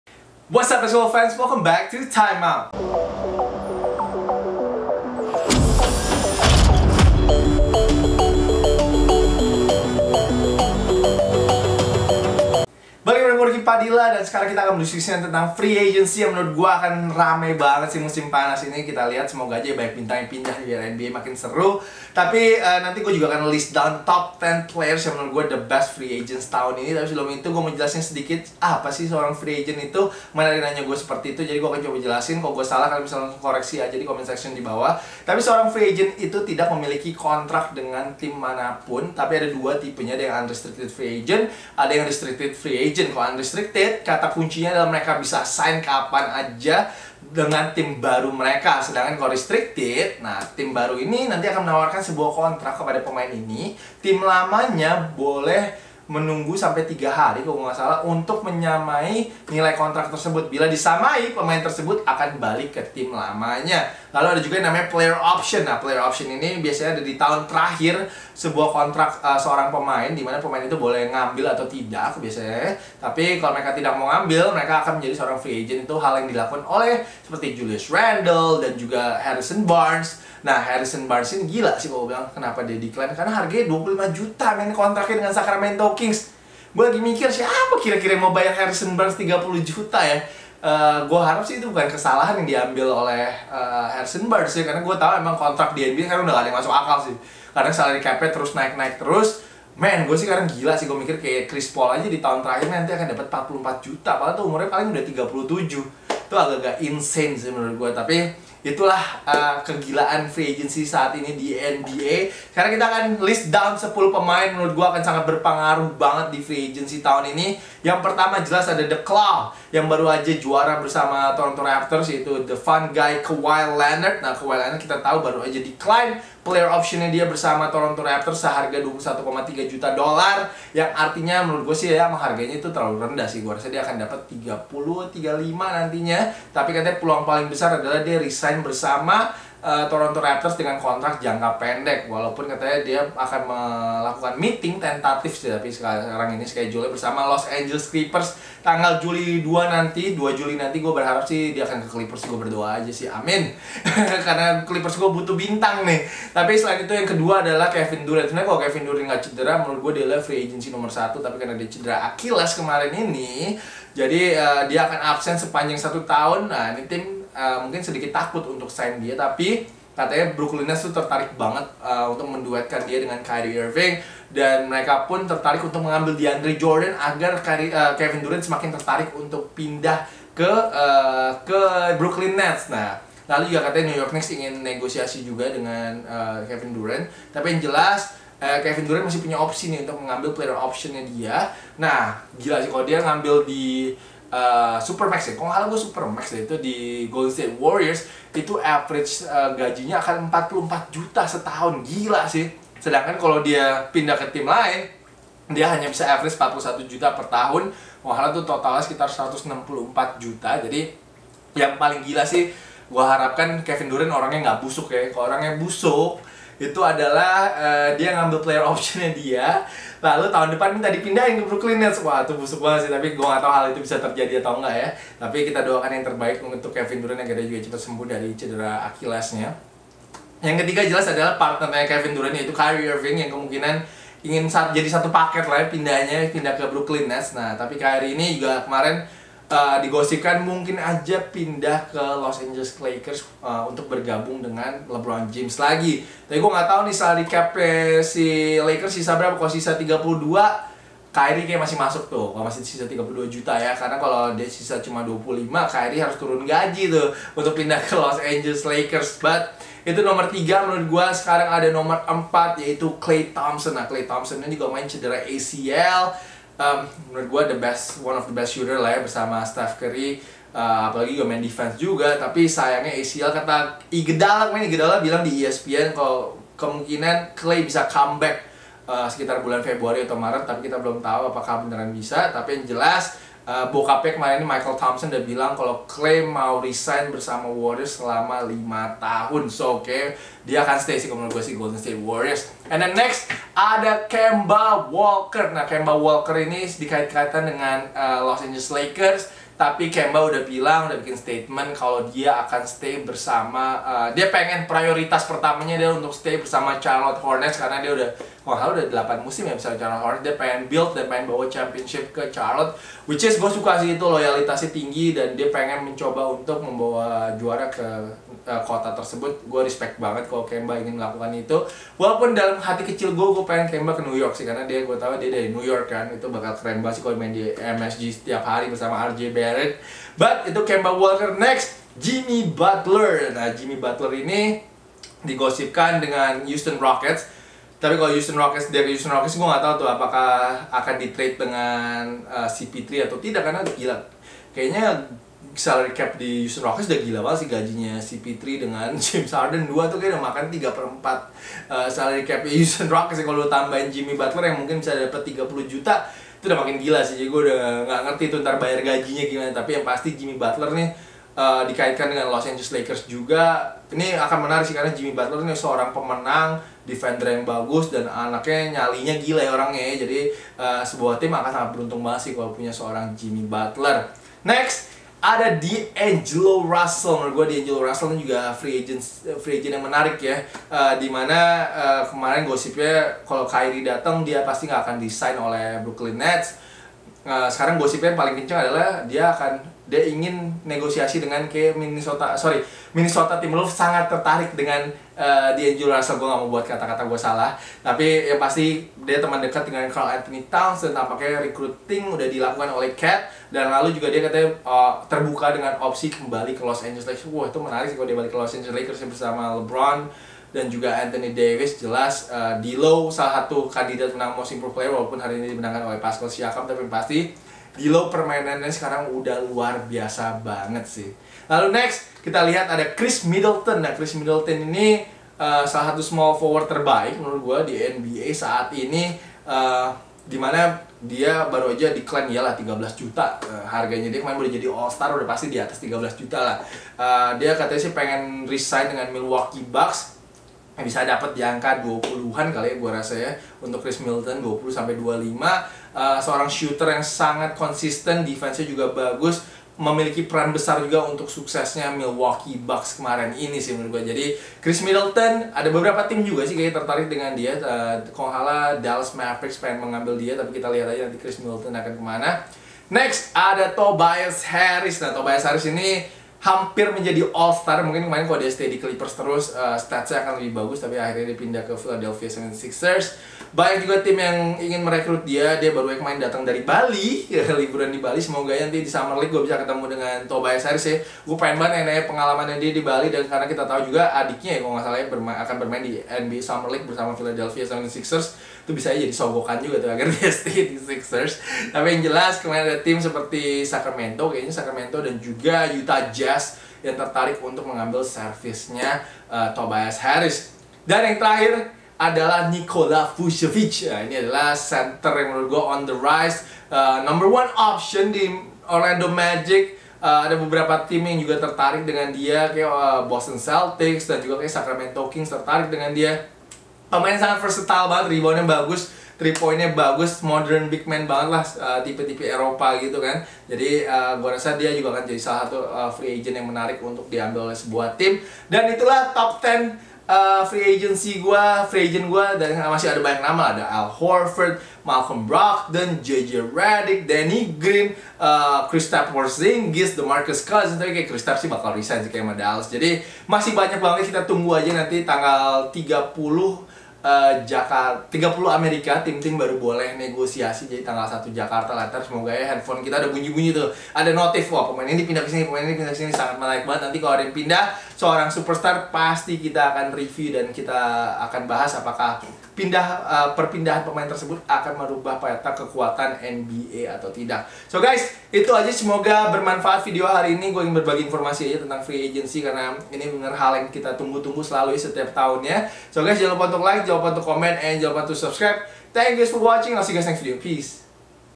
Time Out #280: Top 10 Free Agents Tahun Ini! Bisakah D'Angelo Russell Kembali ke Lakers?
Sharing my top 10 free agents in the NBA this summer.